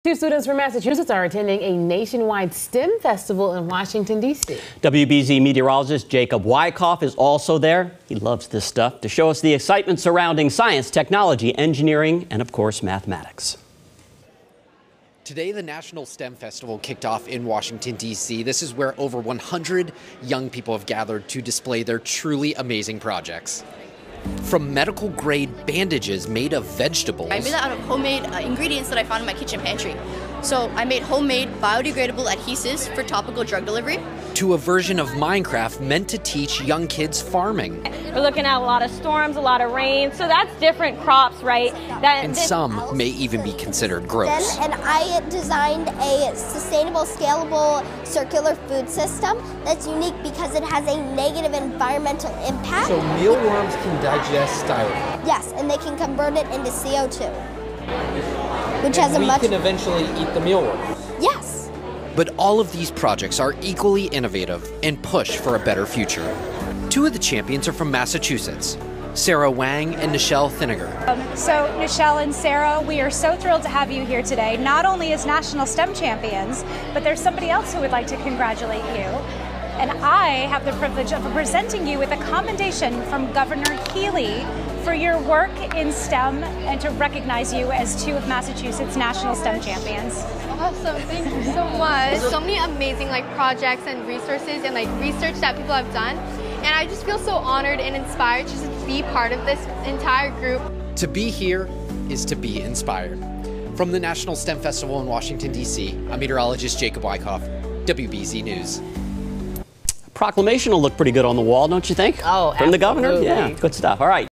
2 Massachusetts students participate in National STEM Festival in Washington, D.C.
Massachusetts students Nichelle Thinagar and Sarah Wang are attending a nationwide STEM Festival in Washington, D.C. WBZ-TV's Jacob Wycoff reports.